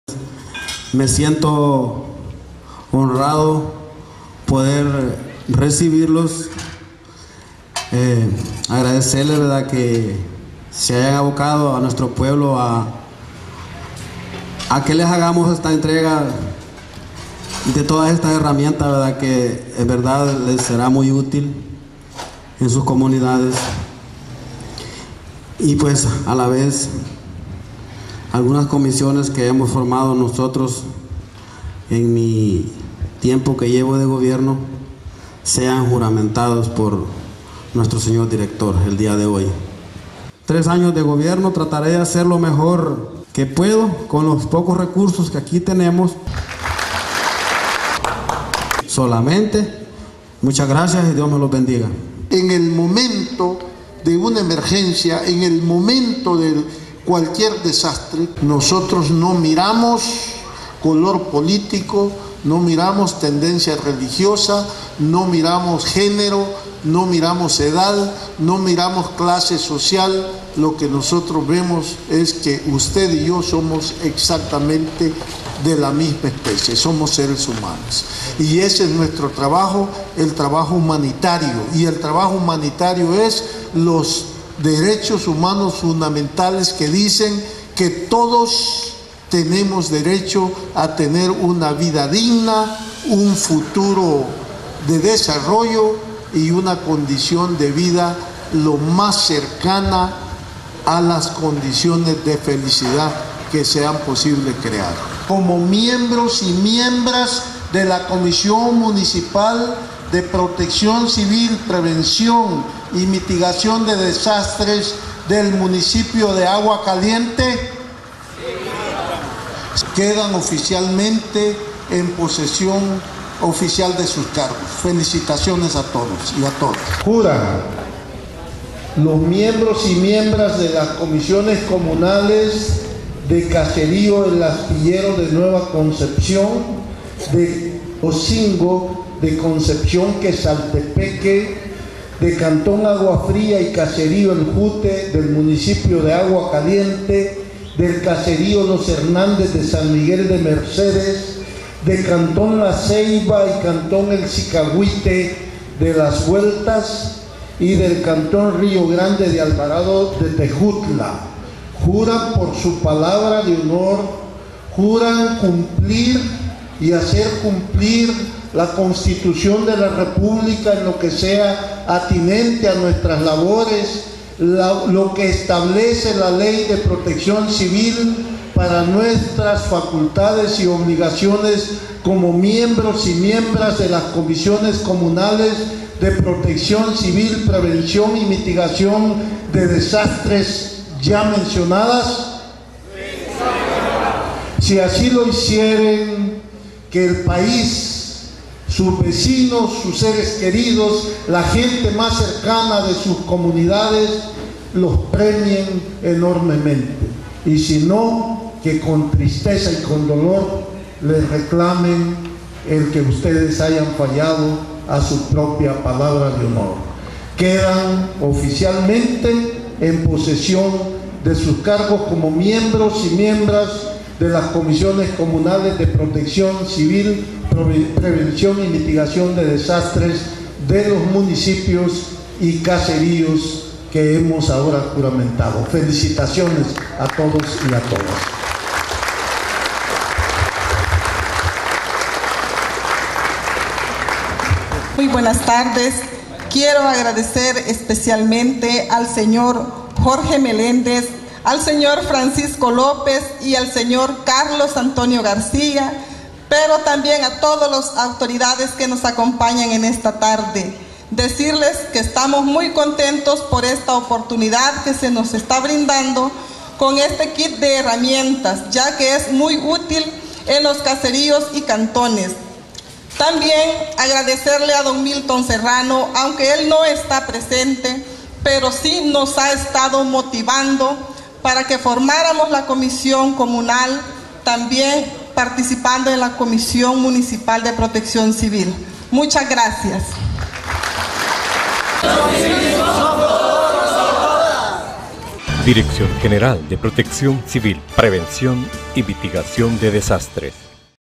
Agua Caliente,Chalatenango 3 dic 15
Director de Protección Civil, juramentó a Comisión Municipal y Comisiones Comunales del departamento de Chalatenango así mismo, hizo entrega de Kit de herramientas básicas para respuesta ante cualquier emergencia.